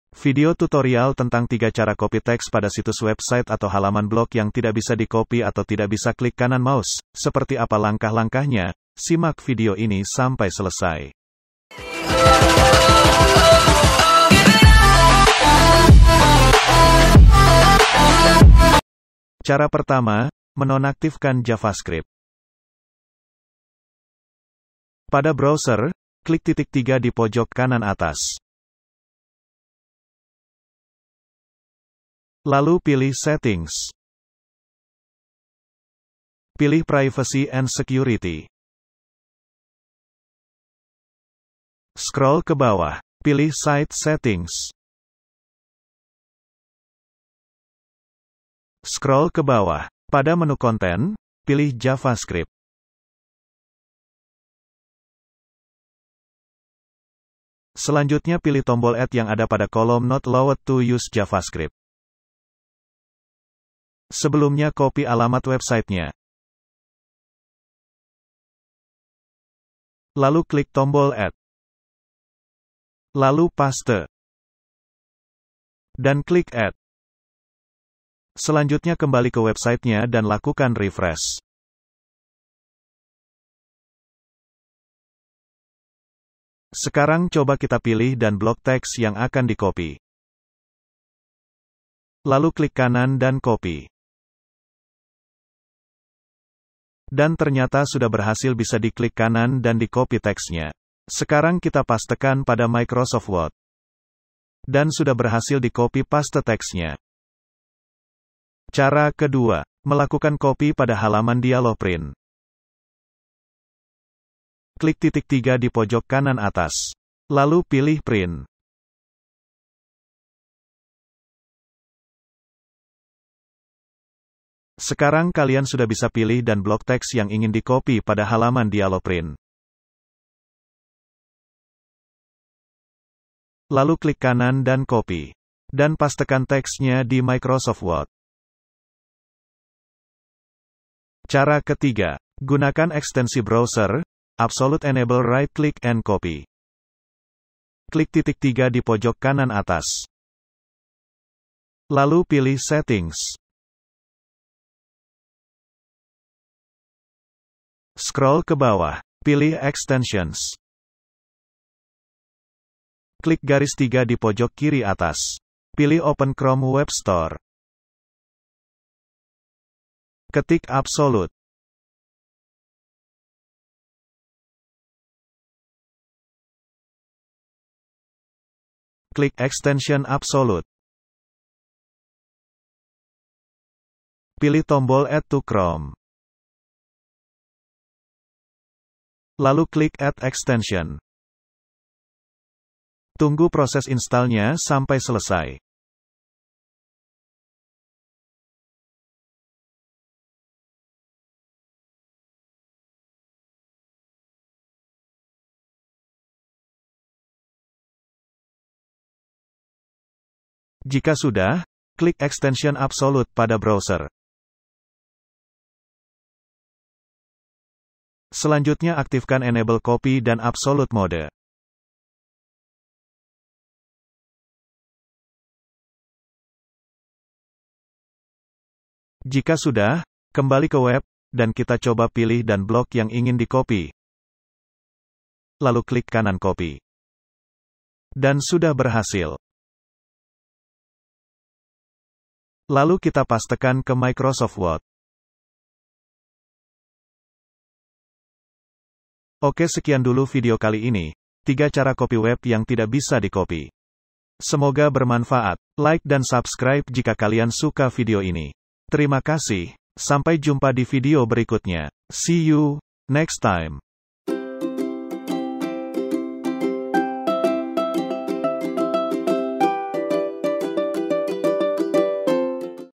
100% BERHASIL!!! Cara Copy Teks di Web Yang Tidak Bisa di Copy
Pernahkah kalian mengalami kesulitan saat mengcopy teks pada halaman website atau blog? bahkan klik kanan mouse pun tidak bisa. ini terjadi karena halaman website atau blog telah dikunci oleh pemiliknya sehingga kontennya tidak bisa di copy oleh orang lain. untuk mengatasinya kalian bisa mengikuti 3 cara yang ada pada tutorial divideo ini :
00:00 Intro
00:20 Menonaktifkan Javascript
01:50 Melakukan Copy Pada Halaman Dialog Print
02:21 Gunakan Ekstensi Browser "Absolute Enable RIght Click And Copy"
04:09 Outro

tutorial cara copy teks di web yang tidak bisa dicopy